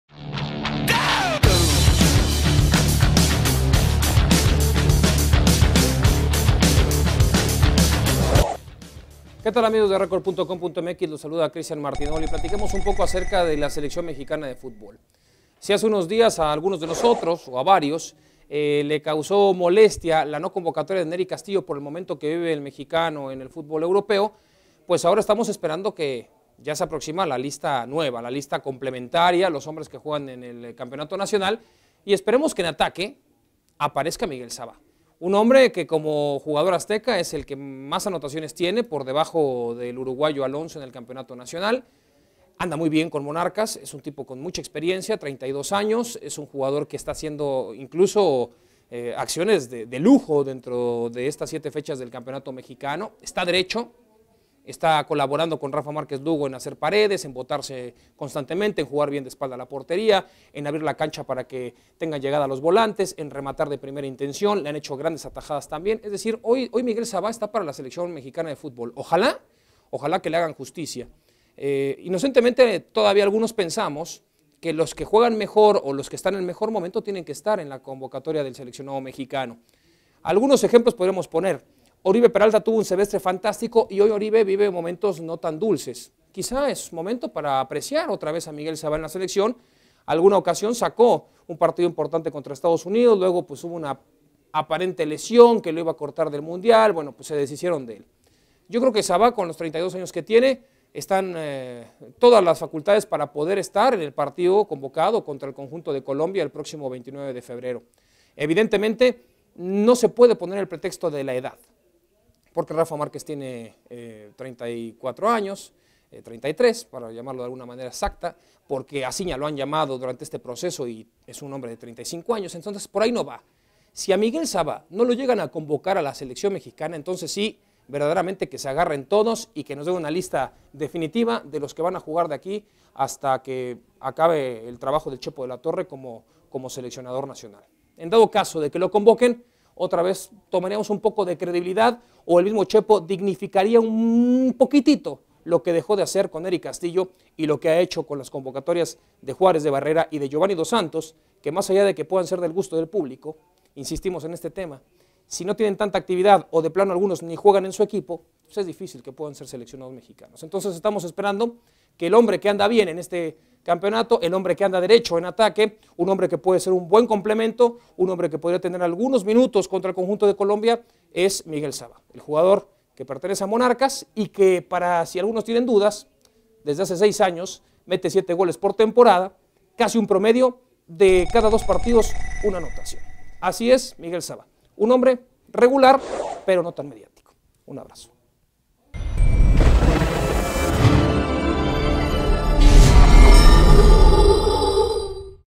Miguel Sabah debe ser llamado al Tri
Christian Martinoli comenta sobre el llamado a la selección nacional, donde destaca a Miguel Sabah como un nombre que tiene argumentos para ser llamado